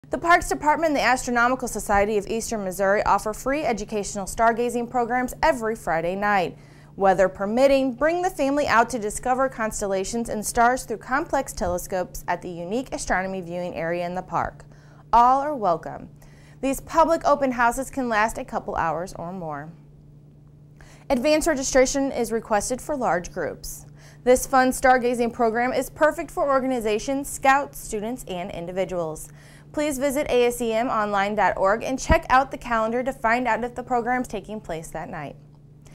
Stargazing Program - Every Friday - Broemmelsiek Park
The Parks Department and the Astronomical Society of Eastern Missouri offer FREE educational stargazing programs every Friday night. Weather permitting, bring the family out to discover constellations and stars through complex telescopes at the unique Astronomy Viewing Area in the park. All are welcome! These public open houses can last a couple hours or more! Advanced registration requested for large groups. This fun stargazing program is perfect for organizations, Scouts, students, and individuals.  Please visit asemonline.org and check out the calendar to find out if the program is taking place that night.